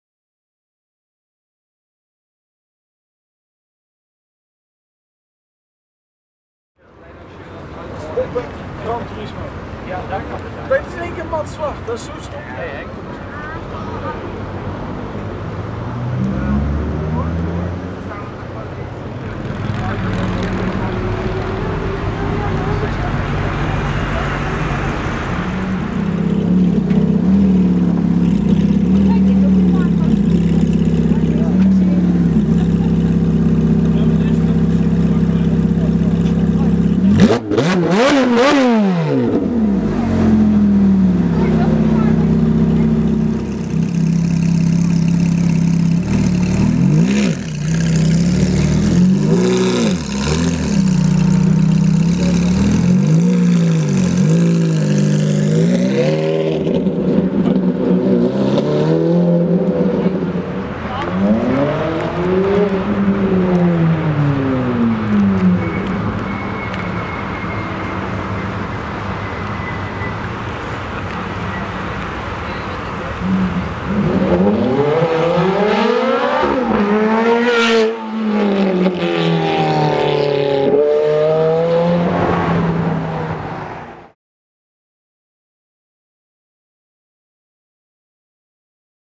Ferrari F430 w/ Capristo Exhaust & Testpipes Sound! LOUD Revs + Acceleration!
I recorded a red Ferrari F430 with a Capristo exhaust system and testpipes at the event Auto Italia Houten 2012. In this video you will see and hear the car revving very loud and do an outrageously fast acceleration. In the acceleration you can really see the front of the car lifting off. The loudness of this car was really out of this world!

What you do think about the car? And what do you think of the sound?

Joost